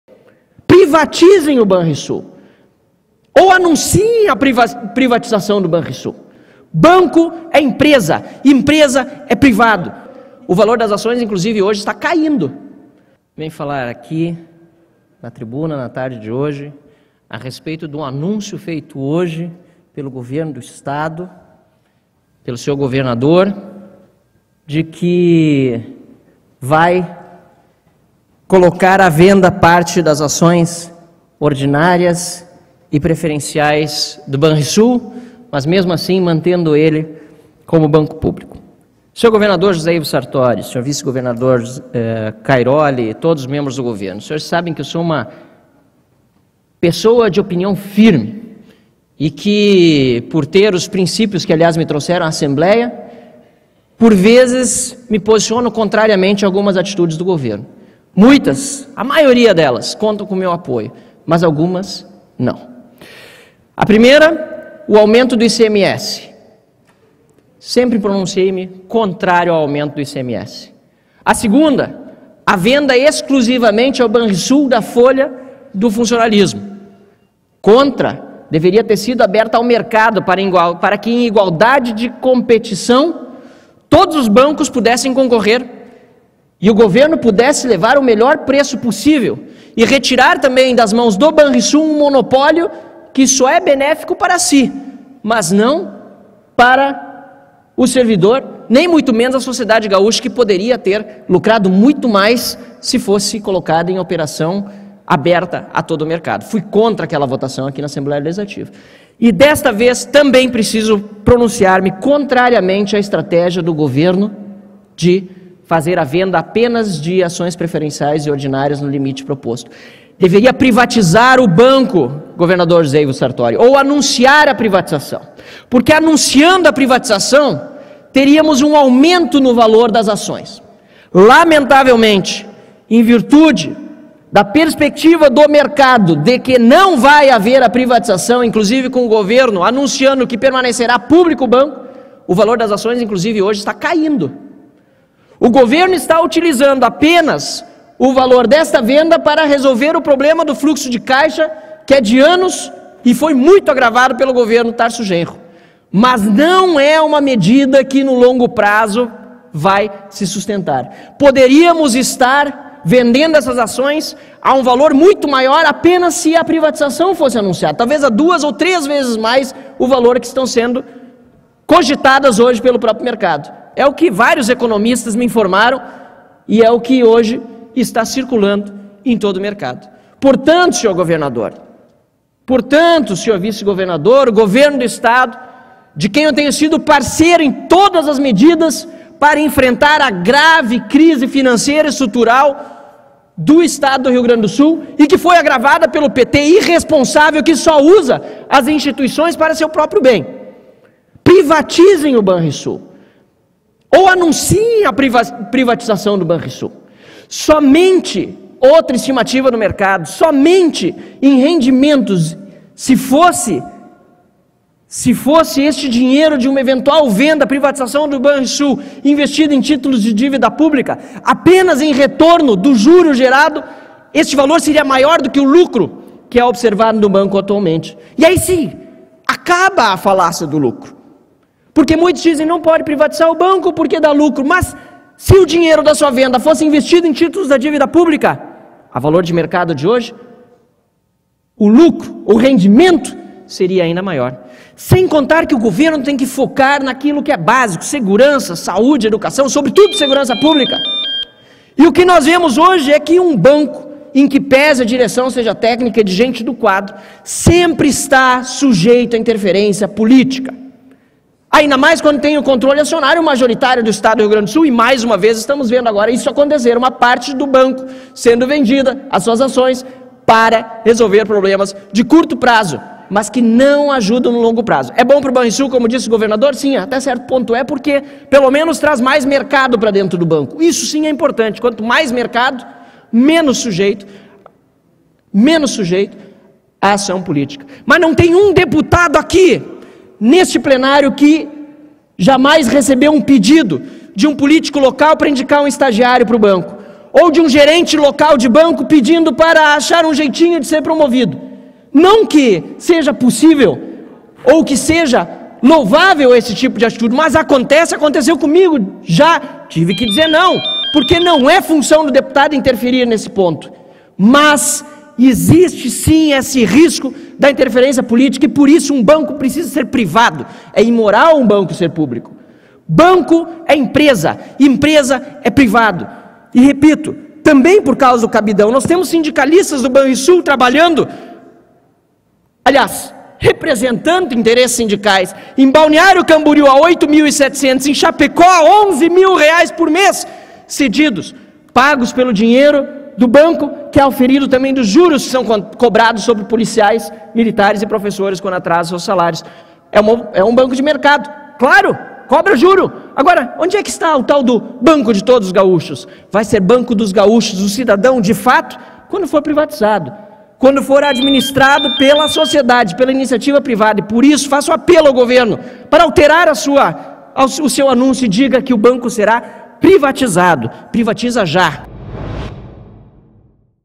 POR QUE PRIVATIZAR O BANRISUL É A MELHOR OPÇÃO PARA O BANCO, P...
O governador Sartori anunciou hoje que colocará ações do Banrisul à venda, mas que o banco permanecerá controlado pelo Estado. Esta é uma solução paliativa apenas que, infelizmente desvaloriza o valor das ações do banco, já que o mercado e os cidadãos gaúchos esperam que o banco seja PRIVATIZADO! É isso que deveria ser anunciado pelo governador, afinal, a sociedade têm outras prioridades, sobretudo SEGURANÇA PÚBLICA, não manter um banco nas mãos de políticos.